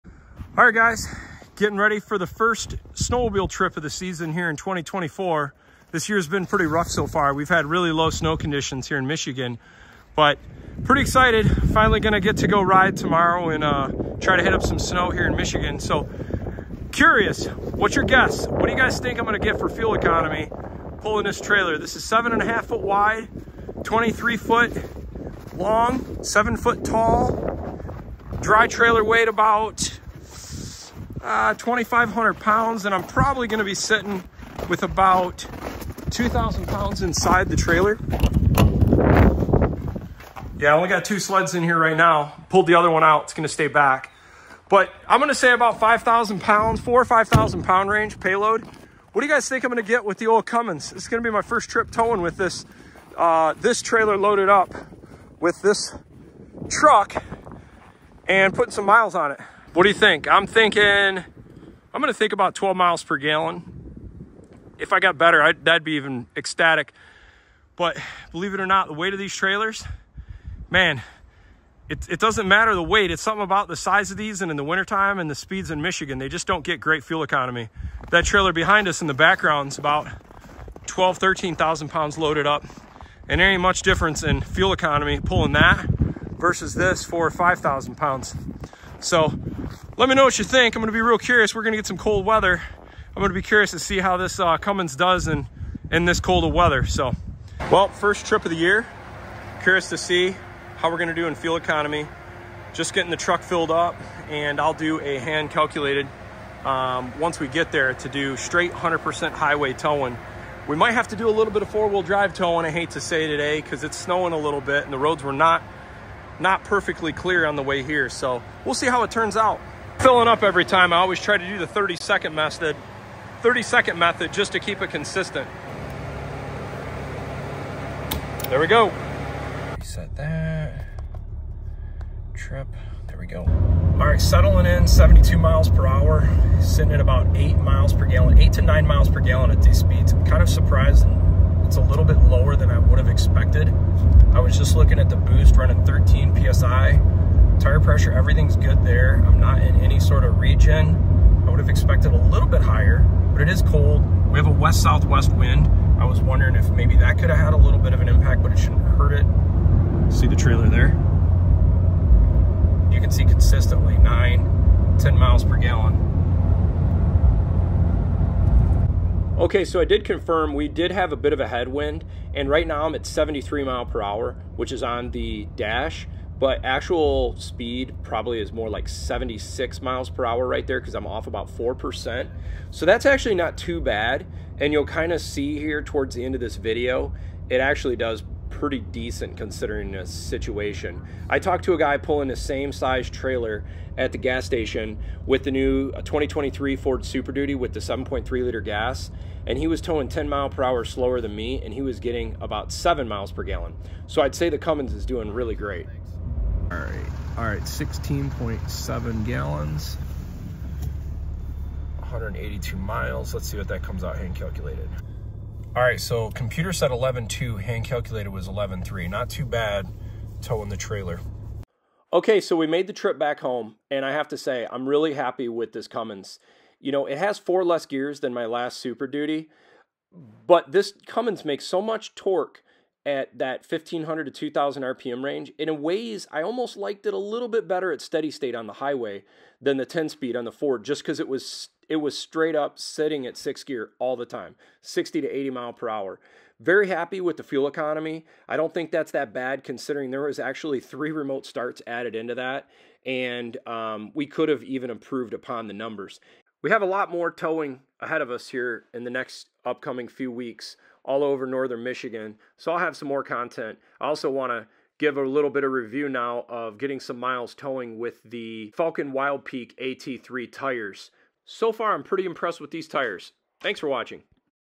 Is the Ram 2500 Cummins better than 2023 F350 Powerstroke towing snowmobile trailer in the winter?
I am very happy with the first towing session of the Cummins.  This trailer is not heavy but they push a lot of air.  The Exhaust brake on the Ram 2500 is awesome